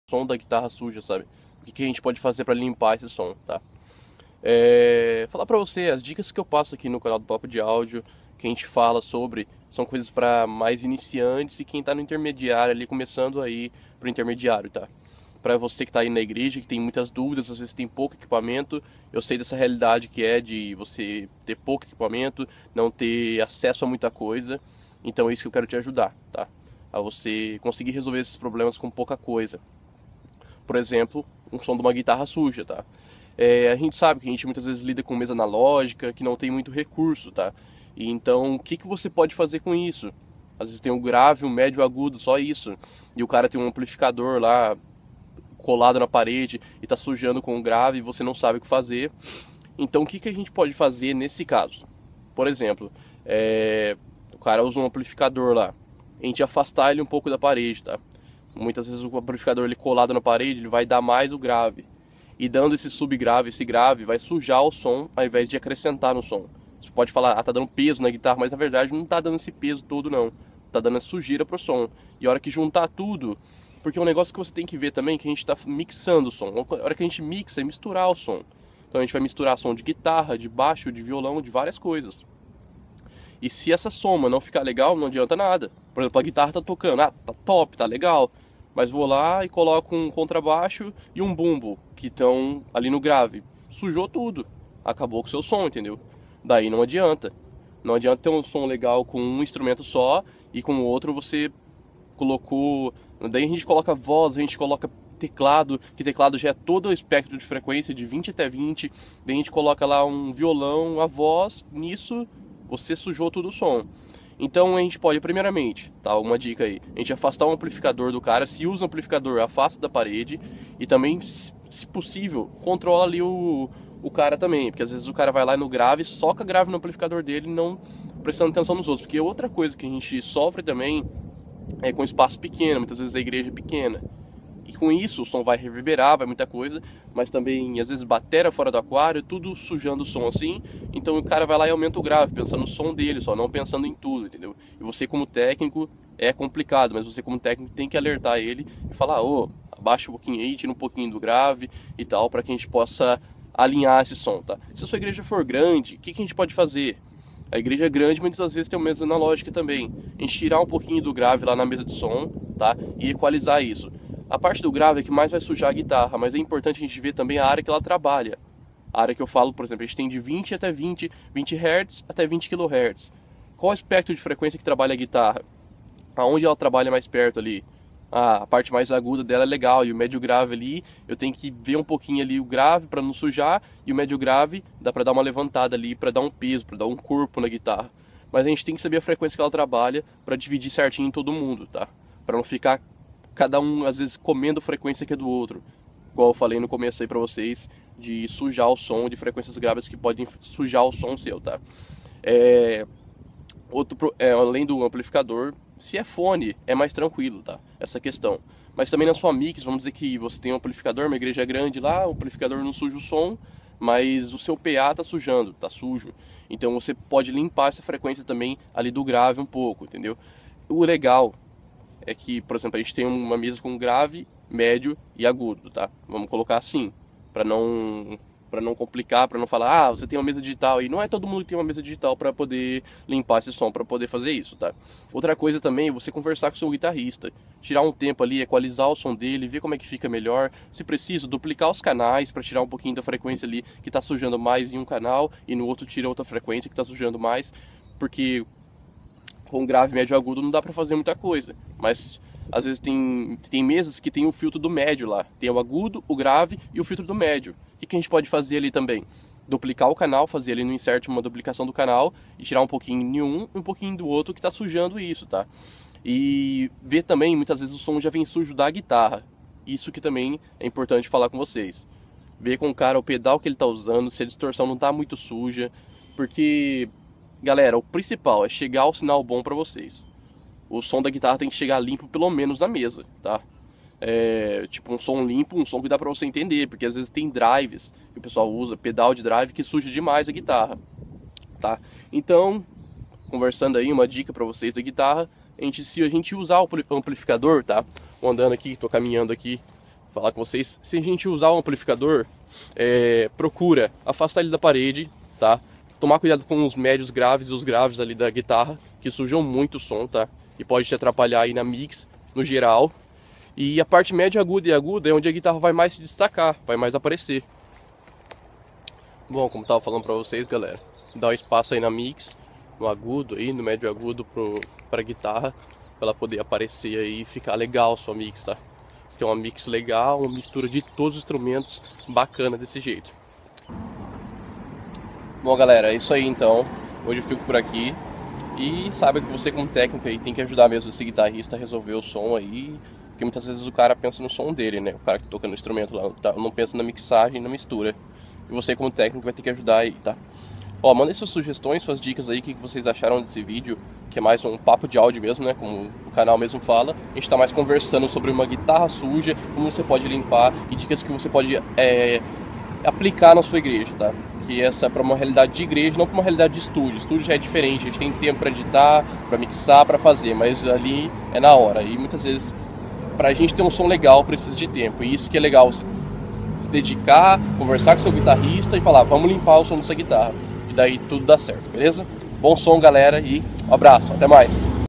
Som de Guitarra na Igreja- Dicas para melhorar seu som.
Fala galera! Bate papo bem legal sobre o Som de uma guitarra, foco principal nesse vídeo é para o pessoal que opera o áudio na Igreja, as dificuldades que pode enfrentar e algumas dicas de como pode ser resolvido os problemas que possivelmente possam ocorrer, precisamos pensar que como técnicos estamos fazendo uma "Mix", isso quer dizer que estamos Misturando vários sinais de áudio, e isso tudo terá um resultado, se vai ser bom ou ruim depende do "Misturador" ( Aquele que vai fazer a mixagem). O som da guitarra é muito bacana para uma música, atualmente temos a guitarra muito presente nas músicas, por isso é importante definir bem o seu som, para que junto com toda banda esteja em harmonia. A banda toda precisa de atenção e cuidado, mas hoje vamos falar sobre a guitarra.
Bom som Galera!